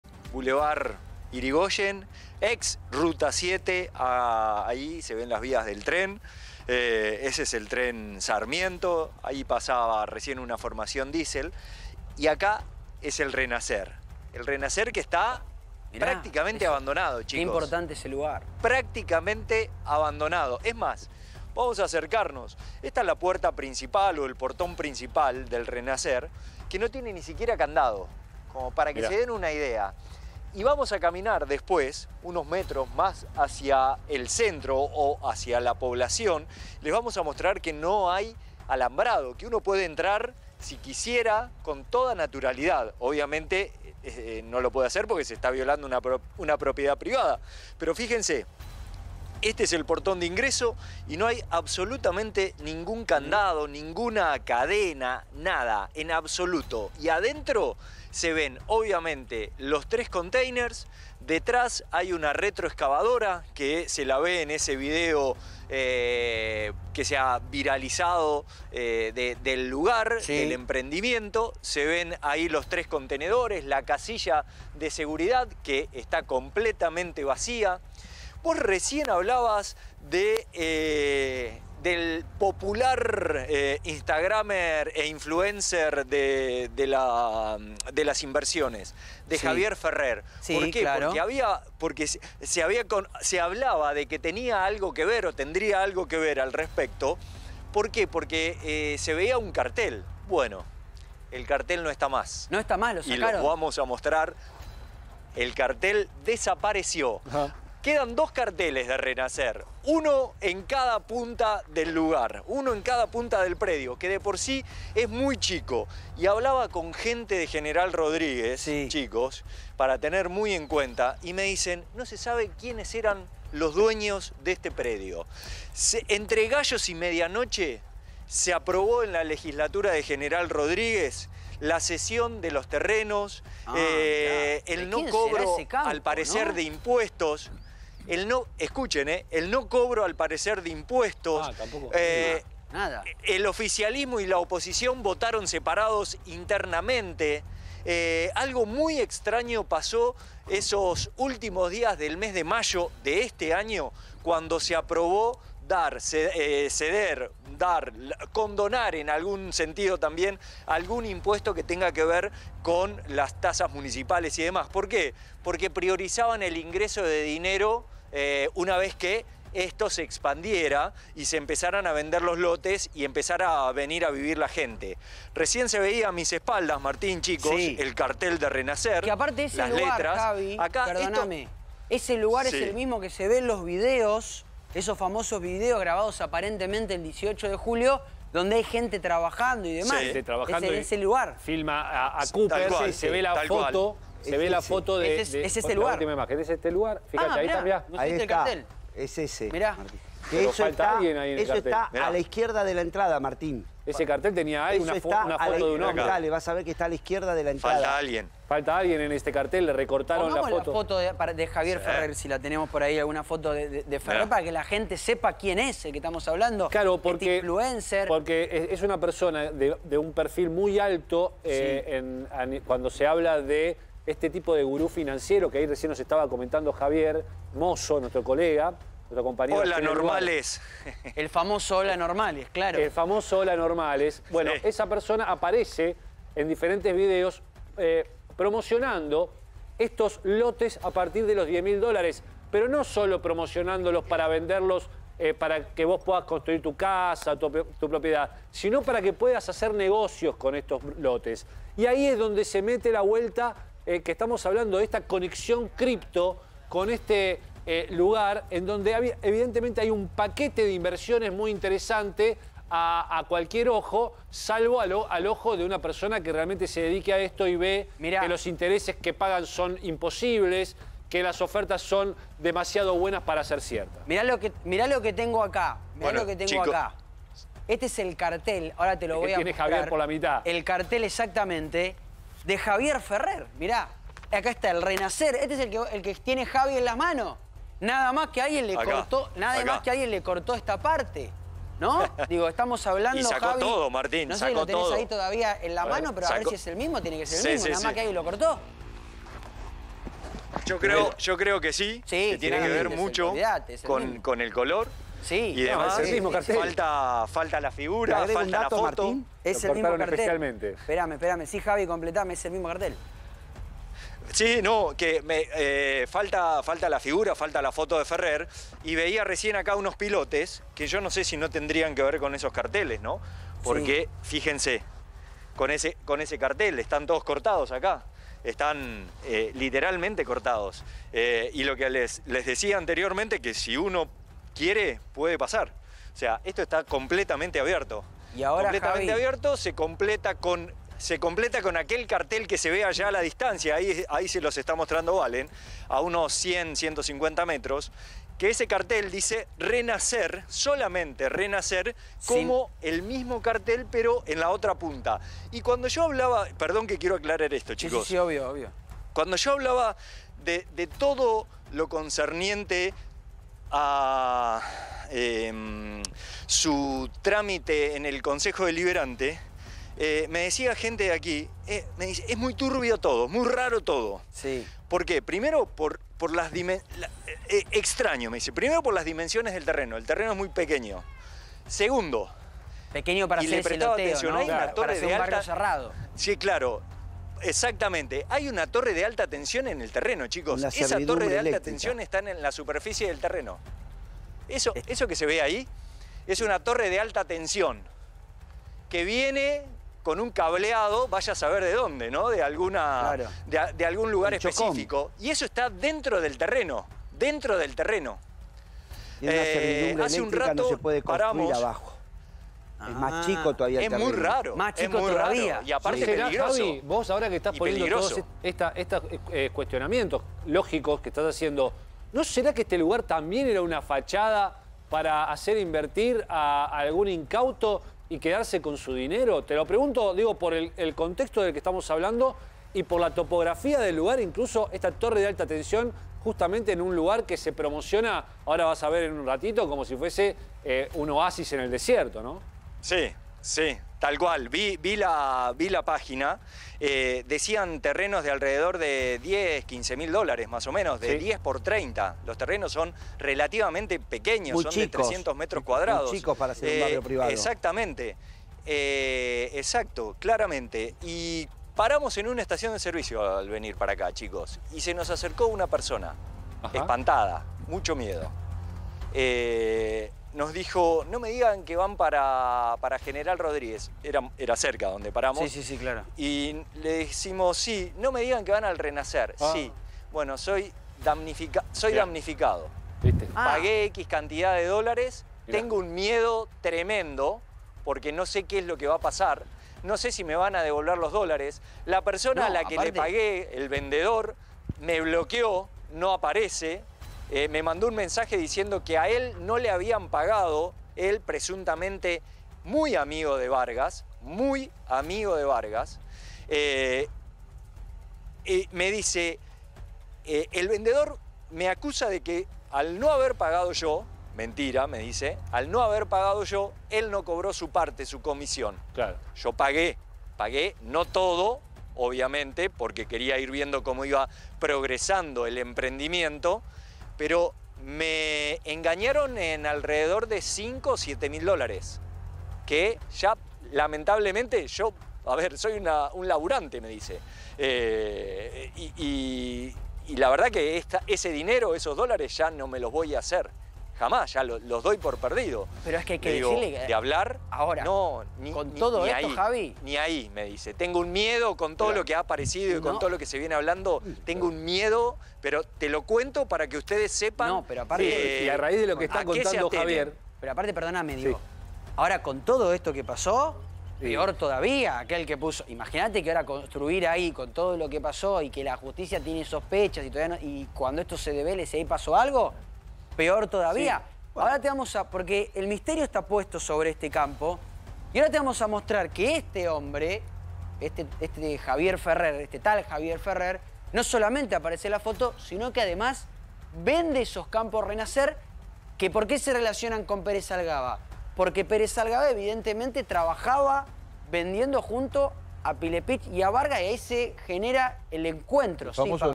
🔴 General Rodríguez: acá "LECHUGA" hizo su último negocio
Continúa la investigación en relación al empresario Ferando Pérez Algaba.